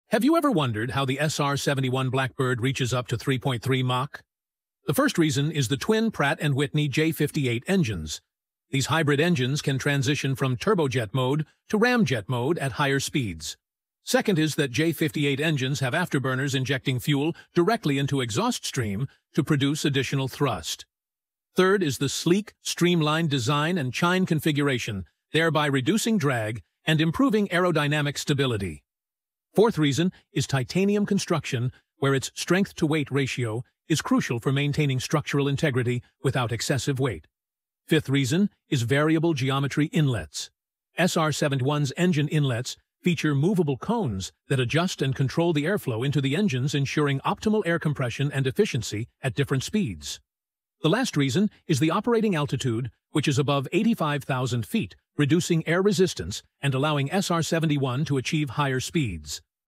What makes SR 71 Blackbird so fast ? Top 06 reasons explained
What makes SR 71 so fast, following are the reasons :-
1. Pratt & Whitney J58 Engines
2. Afterburners
3. Sleek, Streamlined Design with Chines
4. Titanium Construction
5. Variable Geometry Inlets
6. Operational Altitude

Follow me for more such interesting Vides
Lets us all make Aviation Easy